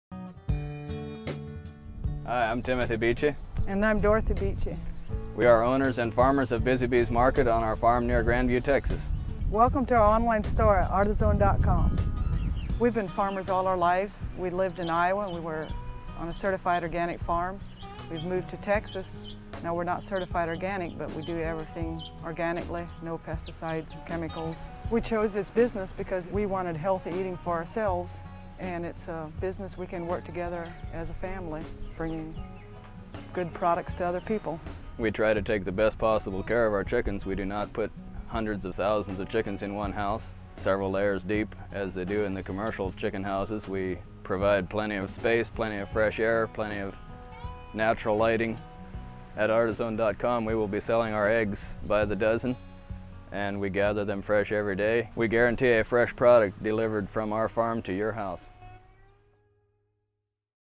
Busy B's Market on Artizone.com
Busy B's Market is run by a young Christian family living on a small farm near Grandview, TX. They moved to Grandview from Iowa, where they ran a certified organic farm. They continue to follow organic farm practices to ensure healthy eating for both their family and yours.